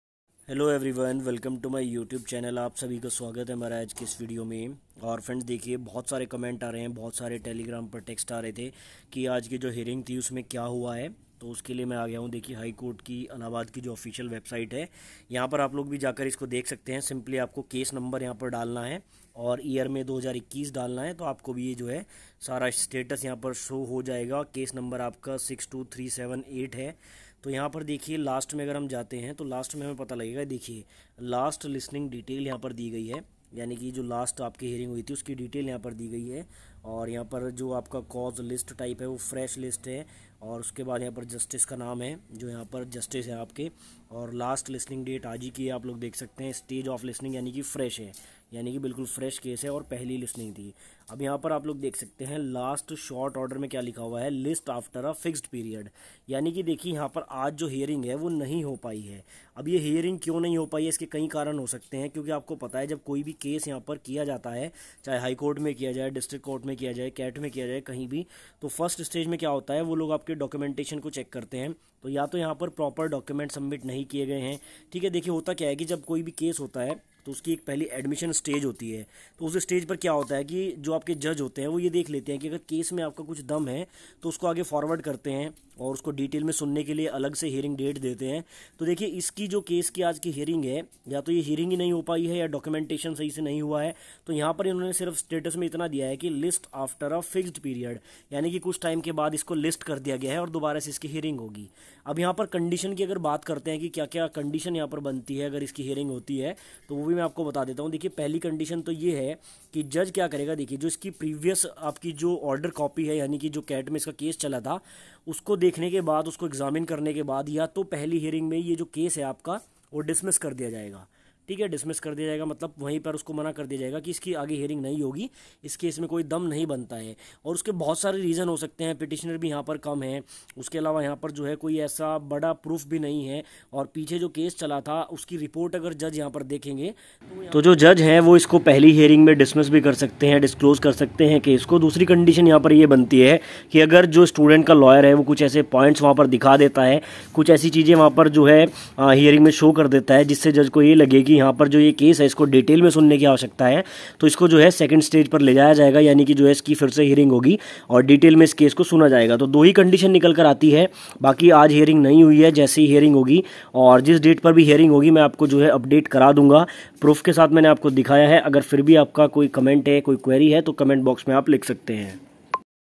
Railway Group-D Exam Date/Group-D Court Case Update/आज की Hearing में क्या हुआ?
Welcome to Exam गुरु  Youtube Channel.